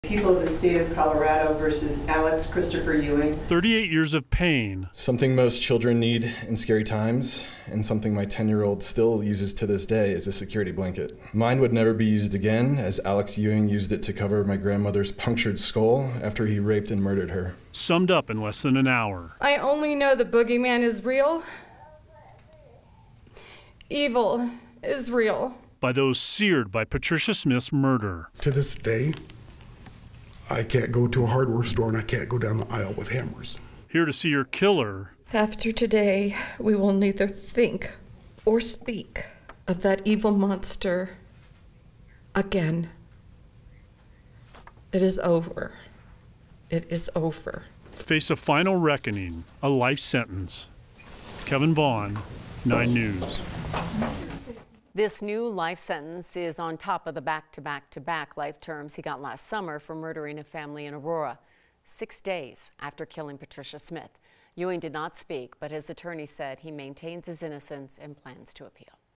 Judge sentences man convicted in 1984 hammer murder
Now serving four consecutive life terms, Alex Ewing is expected to die in prison.

9NEWS (KUSA) is located in Denver, Colorado.